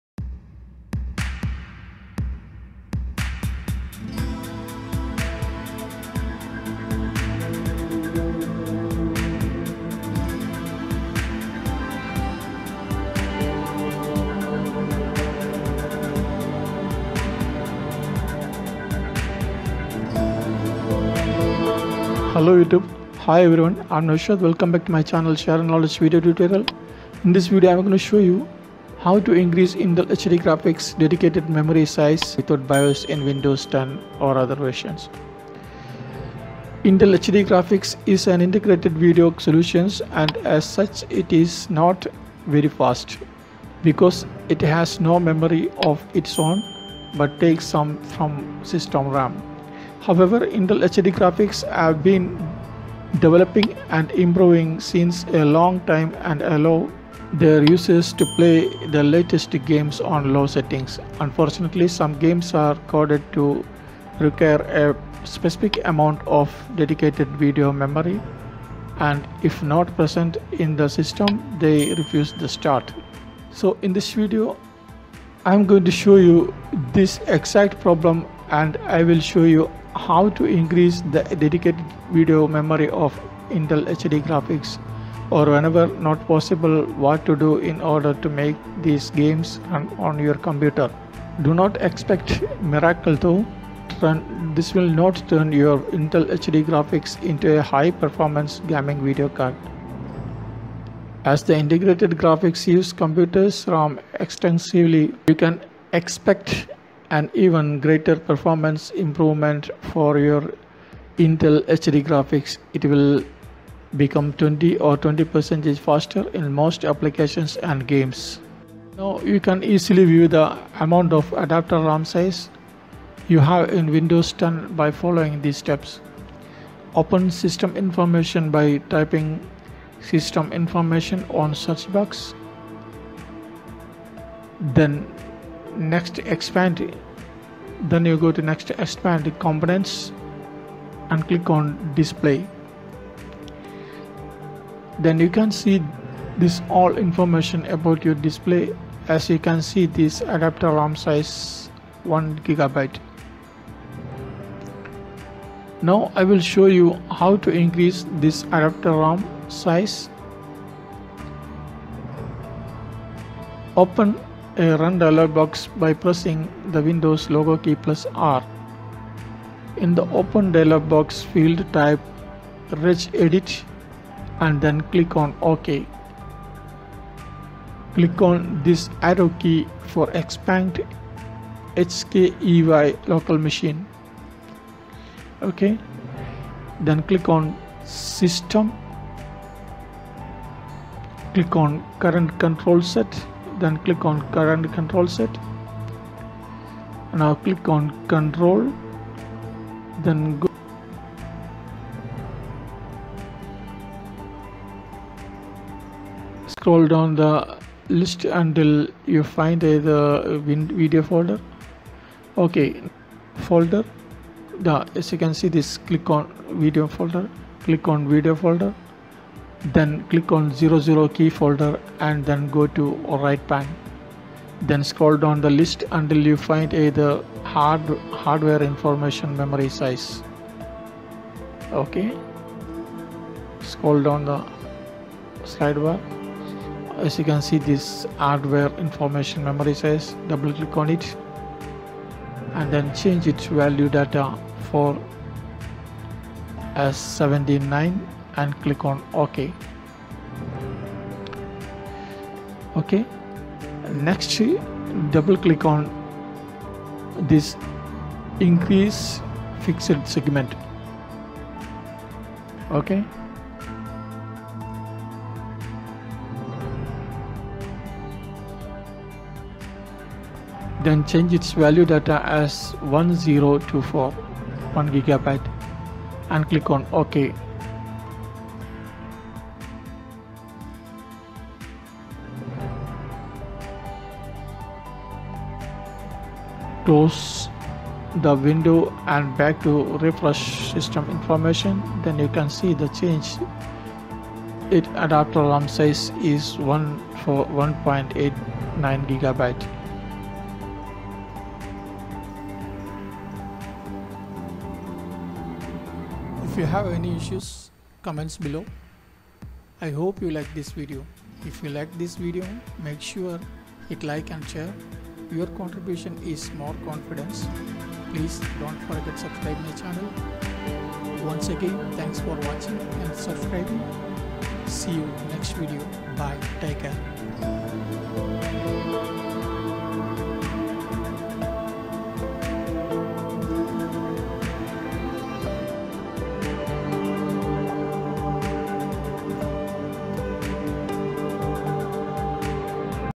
How to Increase Intel HD Graphics Memory Size in Windows10 | How To Increase HD Graphics VRAM Size
How to Increase Intel HD Graphics Dedicated Memory Size in Windows 10|How To Increase Dedicated VRAM

Intel HD Graphics has come a long way and is letting its owners play even the newest games at low settings. But unfortunately, some games are coded to require a specific amount of dedicated video memory and if not present in the system they refuse to start.

In this video I will show you this exact problem and I will show you how to increase the "dedicated" video memory of Intel HD Graphics or whenever it is not possible what to do in order to make these games run on your computer. Do not expect miracles though, this will not turn your Intel HD Graphics into a high-performance gaming video card - it will become 15 - 20% faster in most applications and games.
You can easily view the amount of Adapter Ram Size you have in Windows 10 by following these steps:

Open System information by typing System info on Search Box,
Then Next expand components  and Click On Display
Then you can see this all information about your Display
As you can see this Adapter Ram Size 1 GB
Now I will show you How to increase this Adapter Ram size

Open a Run dialog box by pressing the Windows logo key  +  R.
In the Open dialog box field type "Regedit" and then click on OK.
In the Open Registry Editor window
Click on this arrow key for expand HKEY_LOCAL_MACHINE
Click on System
Click on CurrentConrolSet
Click on Conrol
Click on Video
Then Click On 0000 Key folder and Then go to Right pane
Then Scroll down the list until you find either: (Hardware information Memory size) Double Click On it
Change its value data 40 as 79 and Click On Ok

Next Double Click On (IncreaseFixedSegment)
Then Change its value data as 1024 (1GB) and Click On Ok
Close the window and Back to Refresh System information
Then you can see the change Adapter Ram Size is 1.89 GB
Please share with us your experience by commenting below.. I will definitely replay each and everyone of you as soon as possible

Category
Education
License
Standard YouTube License.If you have some question and suggestions please pm me (Admin)
Regards; (Admin).